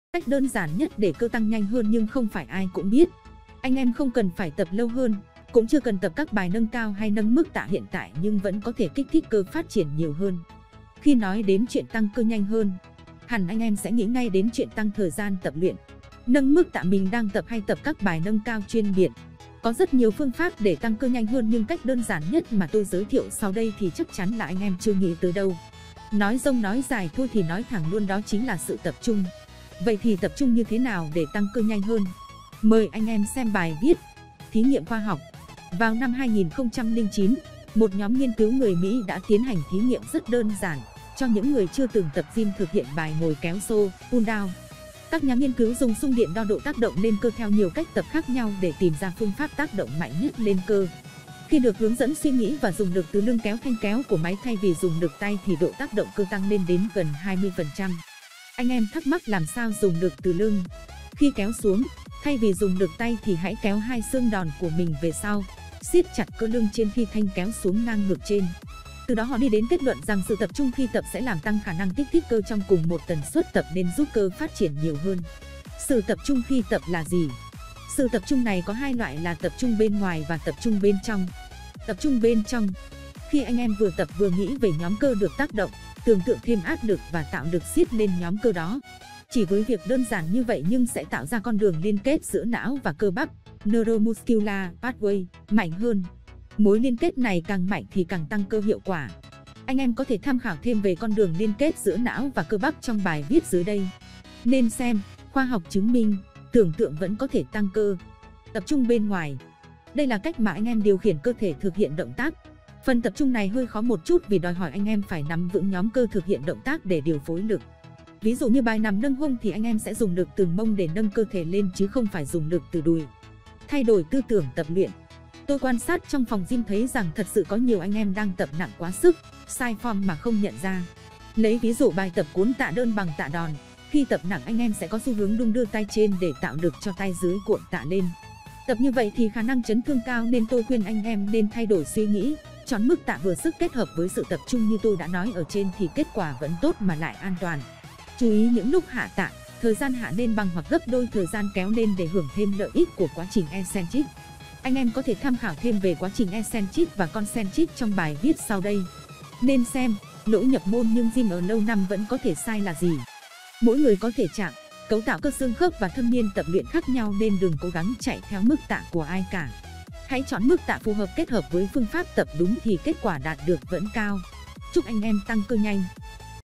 Cát Blog - Cách đơn giản nhất để cơ tăng nhanh hơn nhưng không phải ai cũng biết
Đơn giản nhưng vô cùng hiệu quả nha chiến hữu